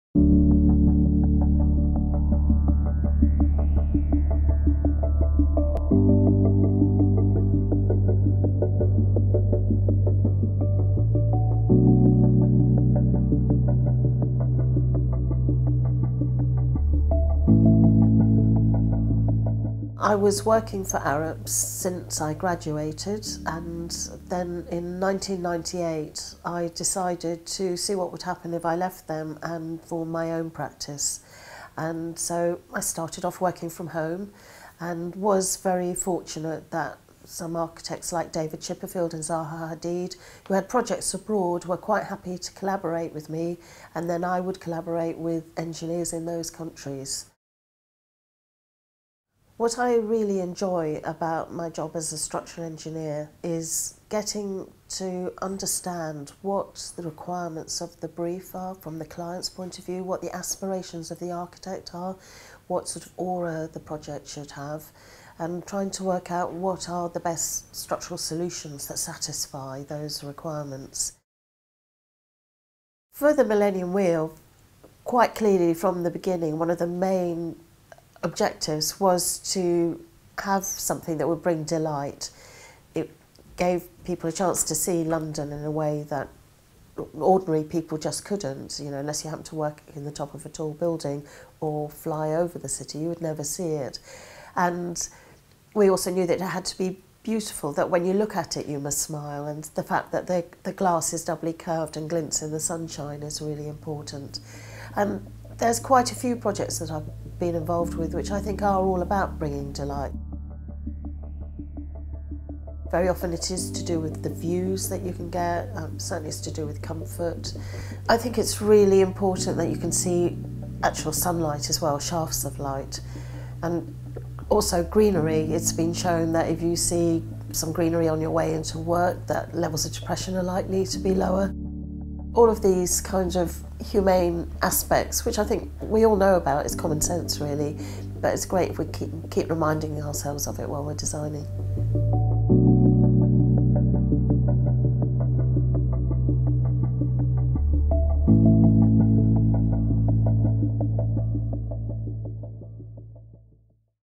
Designing the mighty London Eye: Jane Wernick, Structural Engineer | V&A
Structural Engineer, Jane Wernick, relishes the opportunity to understand the aspirations of the architect, requirements of the client and the aura of the project. The London Eye and the Kew Treetop Walkway both provide aerial views of some of London's best-loved attractions but are also designed to look beautiful, be comfortable, bring delight and make you smile.